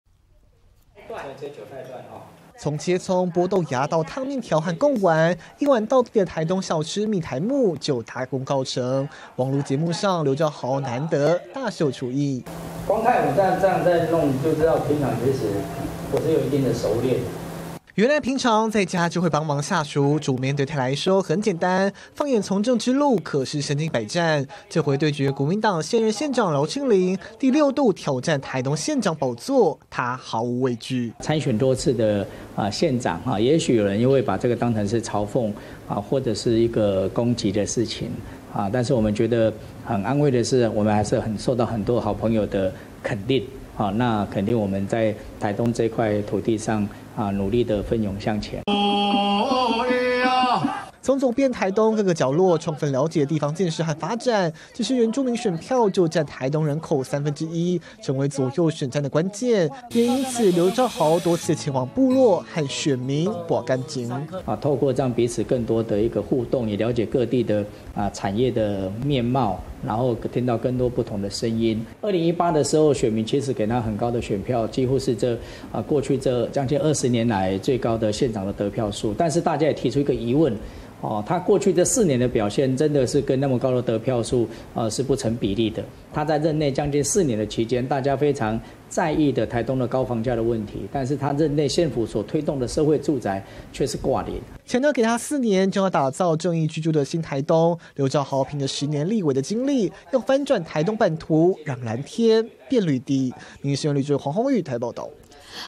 六度挑戰台東縣長 劉櫂豪披綠袍對決饒慶鈴－民視新聞
【民視即時新聞】九合一選舉倒數100天，今年將由劉櫂豪披上綠袍，對決國民黨現任縣長饒慶齡，而這也是他第6度挑戰台東縣長。今天（8月18日）除了拍攝網路節目，大秀料理台東美食米苔目外，也表示給他4年，要打造居住正義的新台東。